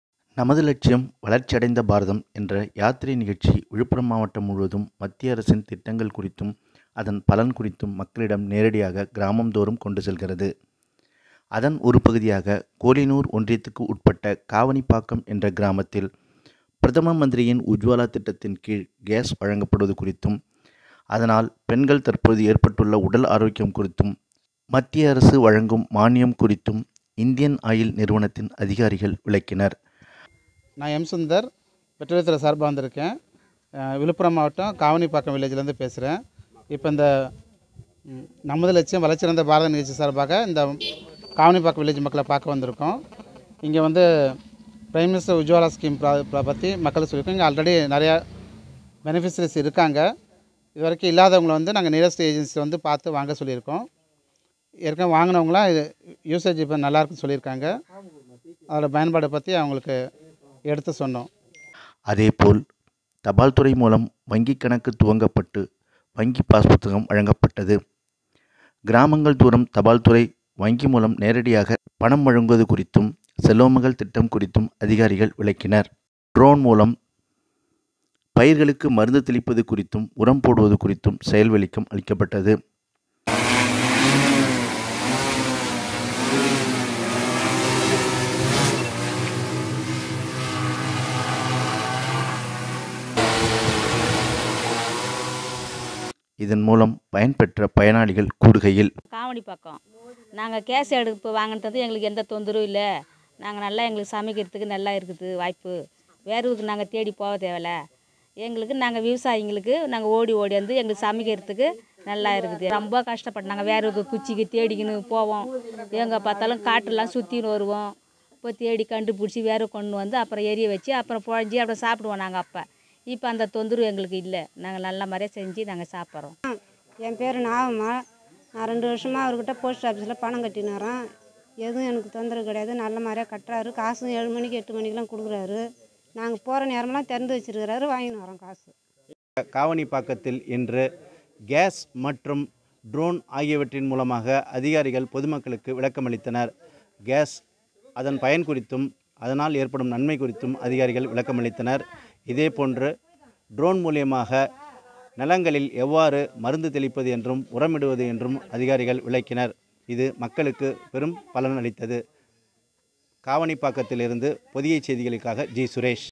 விழுப்புரம் மாவட்டத்தில் மத்திய அரசின் நமது லட்சியம் வளர்ச்சி அடைந்த பாரதம் யாத்திரை நடைபெற்றது
நமது லட்சியம் வளர்ச்சி அடைந்த பாரதம் என்ற யாத்திரை நிகழ்ச்சி விழுப்புரம் மாவட்டம்   காவனிப்பாக்கம் கிராமத்தில் நடைபெற்றது